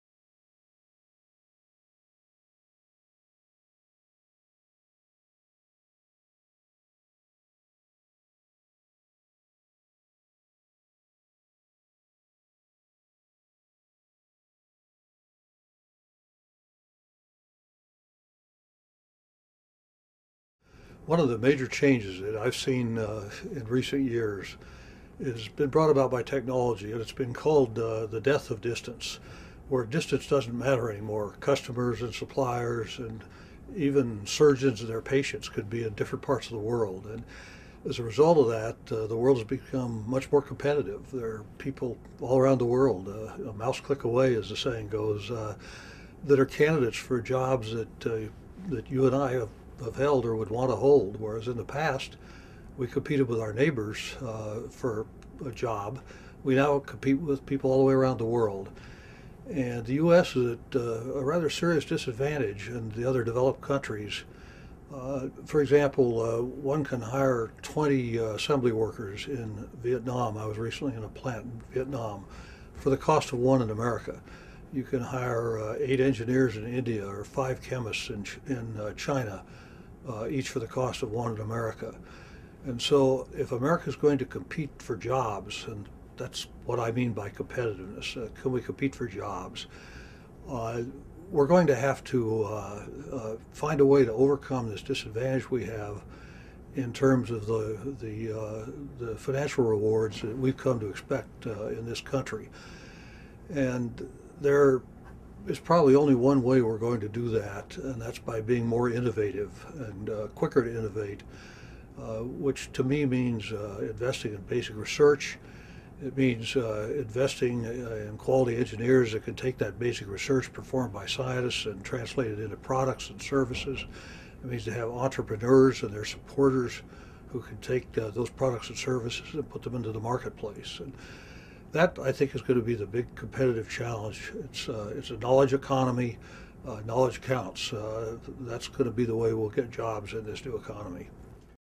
Norman Augustine on U.S. Competitiveness
Norman Augustine, retired Chairman and CEO of Lockheed Martin Corporation, talks about the state of U.S. economic competitiveness.